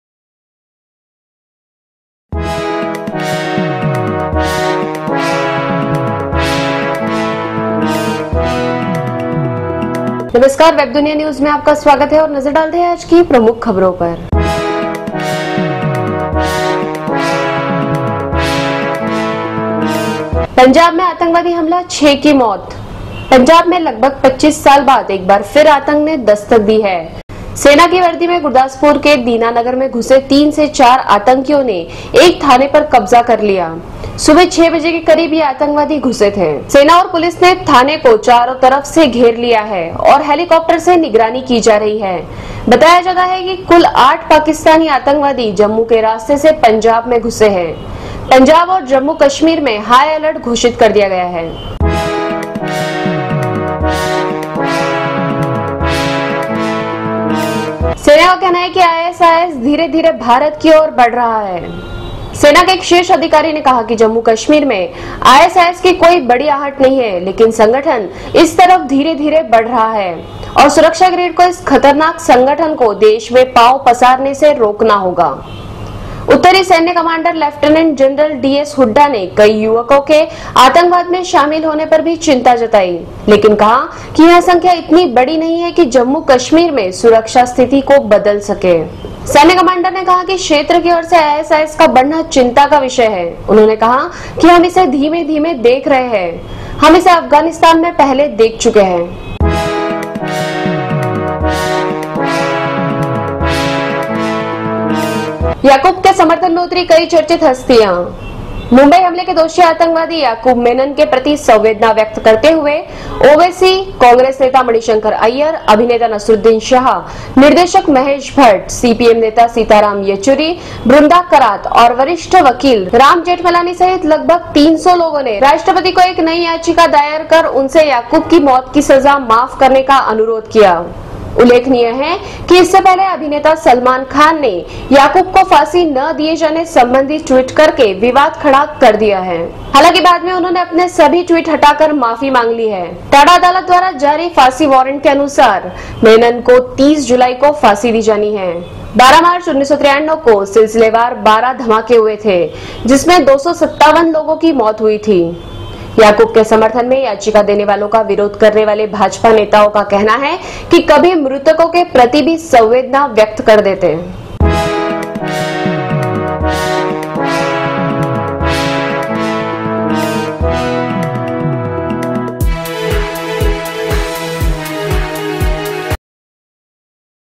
पंजाब में आतंकवादी हमला | Terrorist attack in Gurdaspur
पंजाब में लगभग 25 साल बाद एक बार फिर आतंक ने दस्तक दी है। सेना की वर्दी में गुरुदासपुर के दीनानगर में घुसे 3 से 4 आतंकियों ने एक थाने पर कब्जा कर लिया सुबह 6 बजे के करीब यह आतंकवादी घुसे थे। सेना और पुलिस ने थाने को चारों तरफ से घेर लिया है और हेलिकॉप्टर से निगरानी की जा रही है। बताया जाता है कि कुल 8 पाकिस्तानी आतंकवादी जम्मू के रास्ते पंजाब में घुसे हैं। पंजाब और जम्मू कश्मीर में हाई अलर्ट कर दिया गया है।